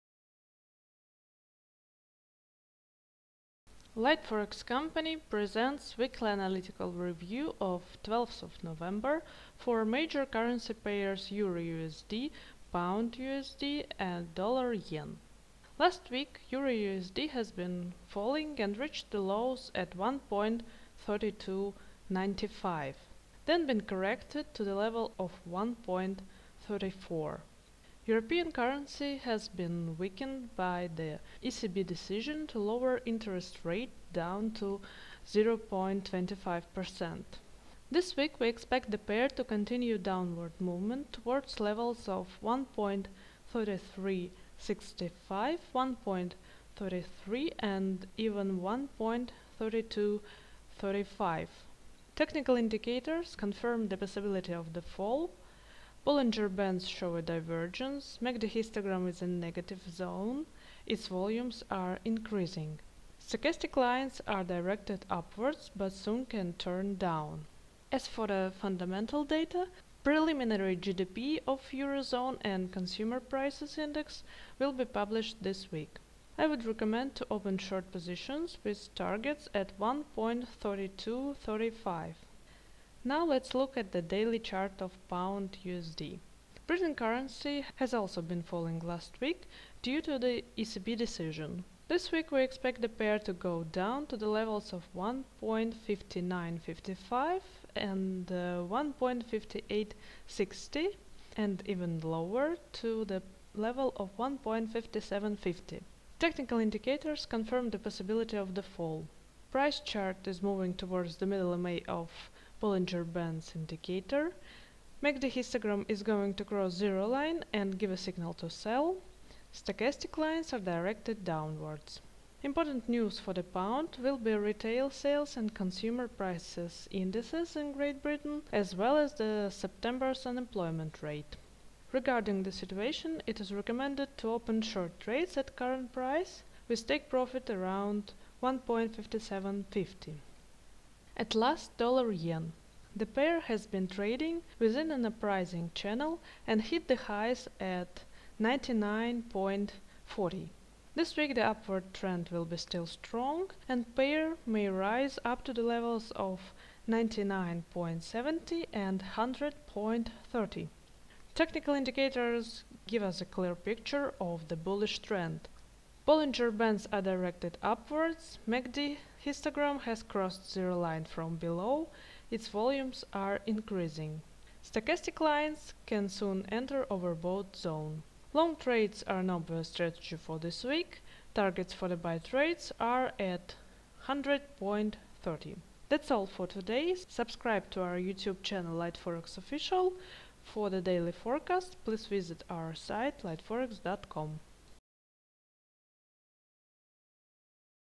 Weekly Forex forecast 11-15 Nov 2013: EUR/USD, GBP/USD and USD/JPY
Weekly fundamental analysis for instruments EUR/USD, GBP/USD and USD/JPY. Daily reviews and forecast are available at LiteForex
Risk Warning: Trading on financial markets carries risks